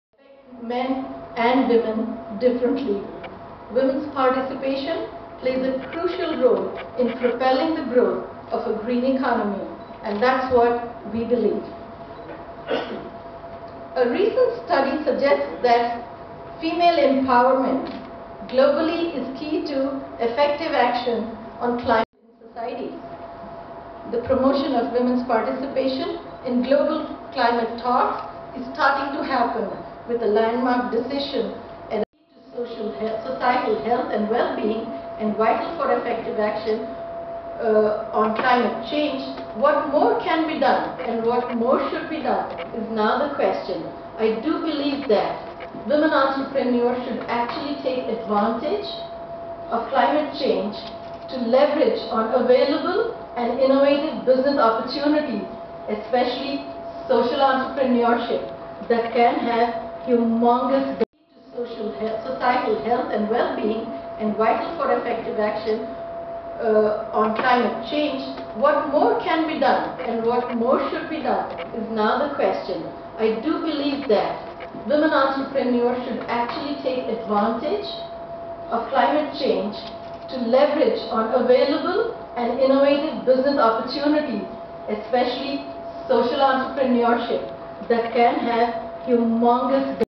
FICCI event at Taj Krishna "Women: Untapped Drivers of Green Economic Growth?
"Women: Untapped Drivers of Green Economic Growth?, attended by FICCI Ladies Organization (FLO) members. Location TaJ Krishna